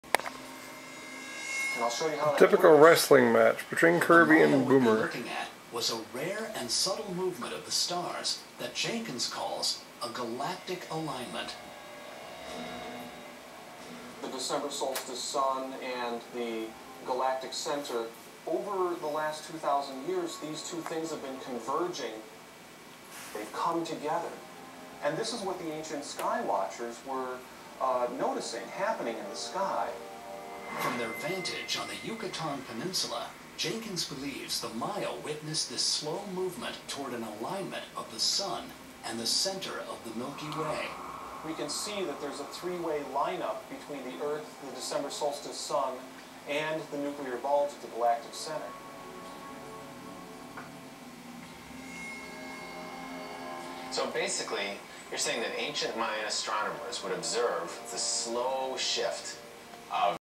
Wrestling cats
Boomer and Kirby doing what they like to do, Wrestle!